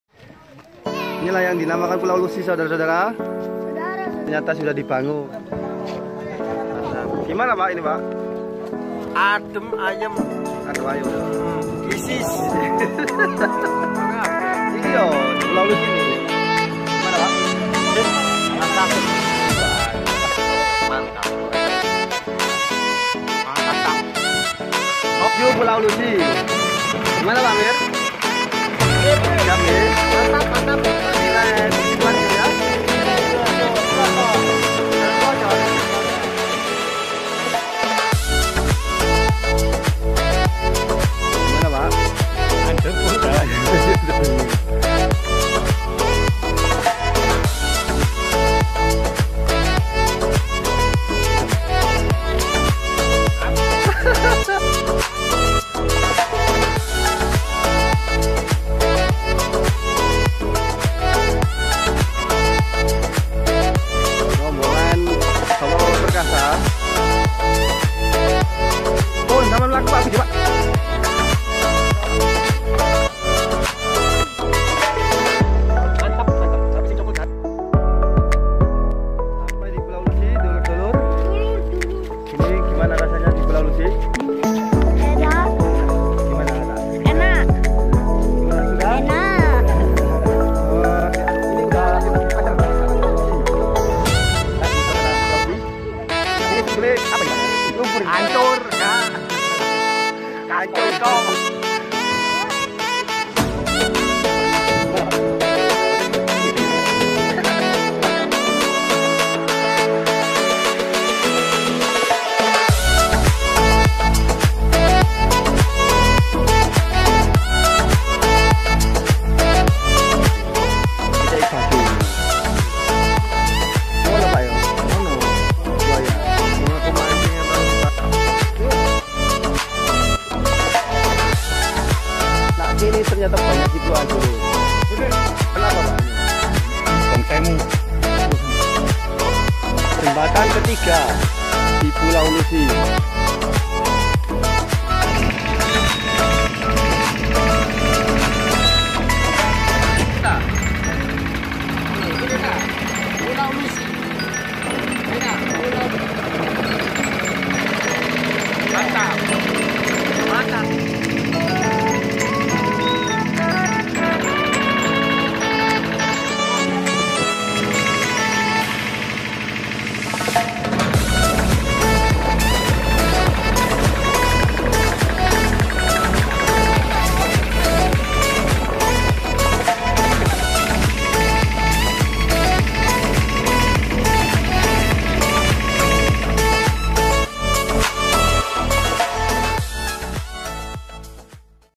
LUSI ISLAND SIDOARJO
Berkunjung di Pulau Lusi milik Sidoarjo bersama Bapak Bapak Perkasa Perumahan Gading fajar 2 Sidoarjo.
Sempat menjadi hantu yg menyelimuti saya , membayangkan seperti apa pulau lusi yg berada di pucuk kota Sidoarjo ini..
Dan akhirnya , tibalah waktu utk berkunjung disana ..
Utk kepulau lusi membutuhkan waktu 30menit utk menyebrang menggunakan perahu milik nelayan warga sekitar ..
Pokoknya seru john ..
Merapat kesana biaya murah sekali ..